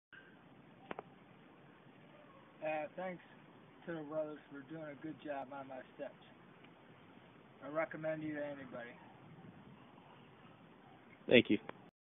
Tittle Brothers Customer Review
Tittle Brothers Construction replaced this customers front steps and railing in Lincoln Park Michigan